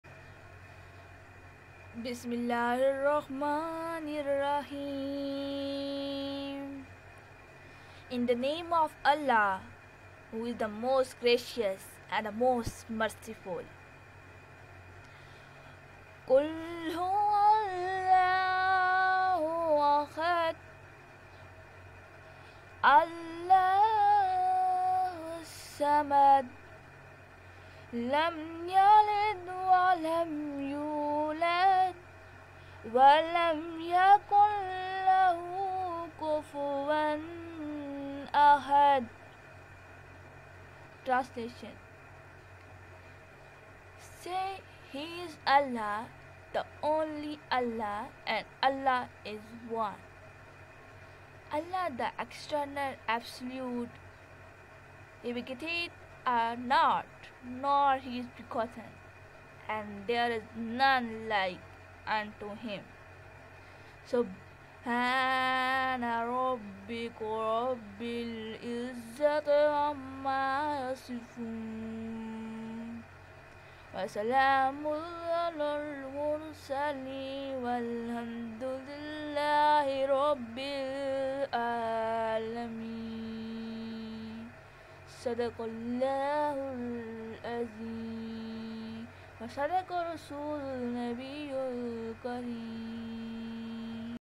The Best Surah Which Give Witness About The Oneness of Allah
"This  is the best and shortest Surah of Holy Book Quran  which Can understand easily but  taking in believe is too tough"